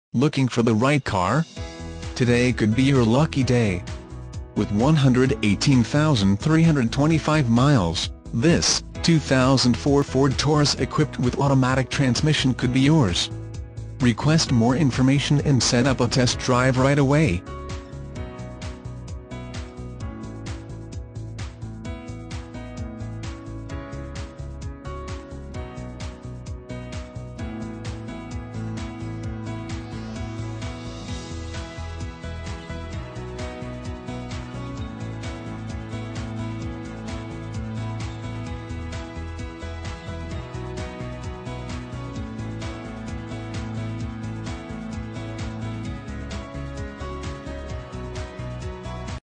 2004 FORD TAURUS Rockford, IL U101556
2004 FORD TAURUS Rockford, IL
Stock

Rock River Block Ford Kia Mitsubishi
224 North Alpine Road
Rockford, IL

Looking for the right car? Today could be your lucky day. With 118325 miles, this , 2004 Ford Taurus equipped with automatic transmission could be yours. Request more information and set up a test drive right away.